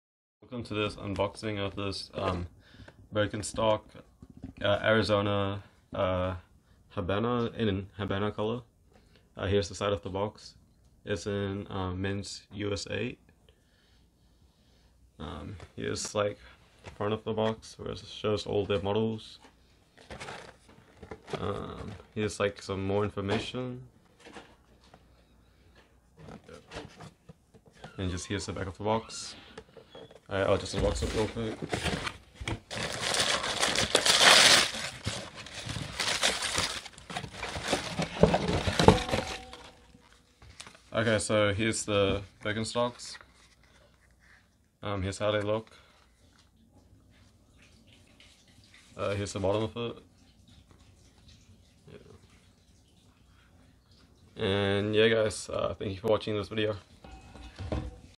Birkenstock Arizona Sandal Unboxing and First Look
Product unboxing of Birkenstock Sandal model Arizona made in Germany.

video